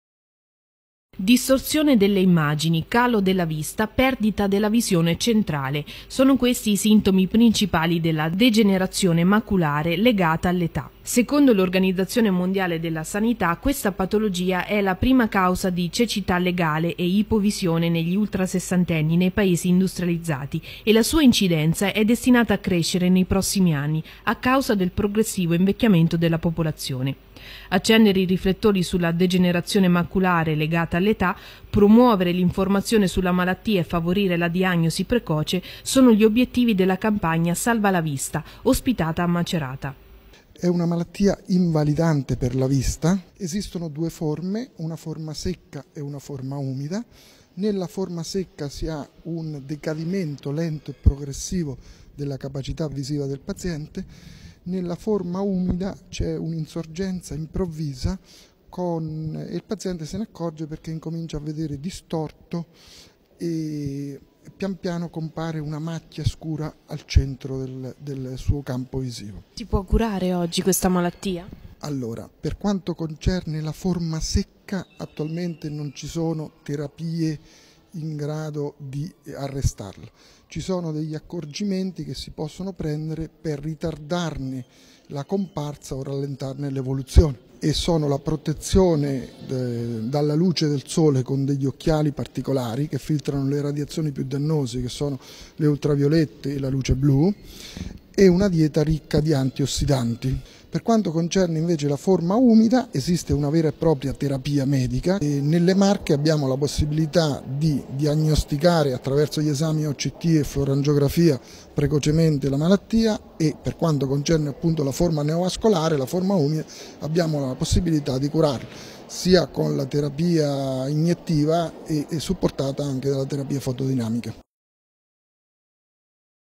MACERATA. CAMPAGNA SALVA LA VISTA
Macerata. 2 novembre 2011. Degenerazione maculare legata all'età : a macerata una campagna di informazione e sensibilizzazione per la difesa della vista da questa patologia poco conosciuta ma che rappresenta la prima causa di cecità legale nei paesi occidentali. Servizio di Stefania Rossetti.